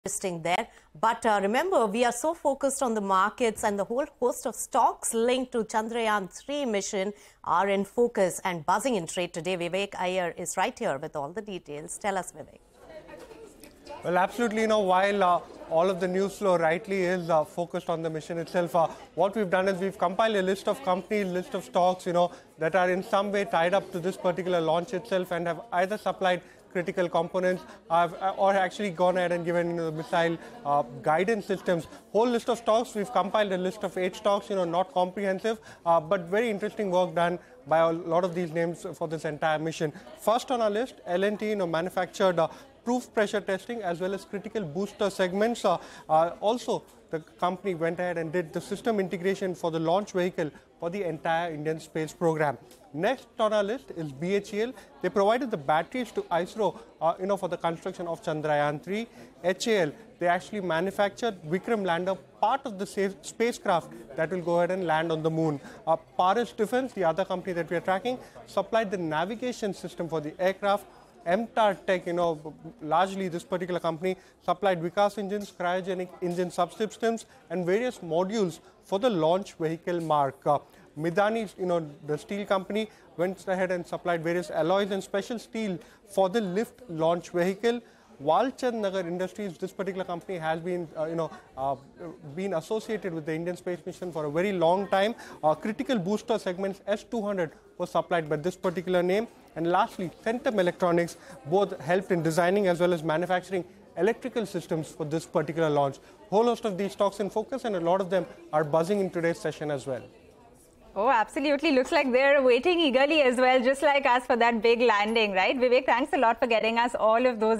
Ahead Of Chandrayaan-3’s Landing On The Moon, A Number Of Supplier Stocks Are Buzzing | CNBC TV18
As India nears a historic feat, a whole host of stocks linked to Chandrayaan-3 are buzzing in trade today. Vivek Iyer with the details.

n18oc_business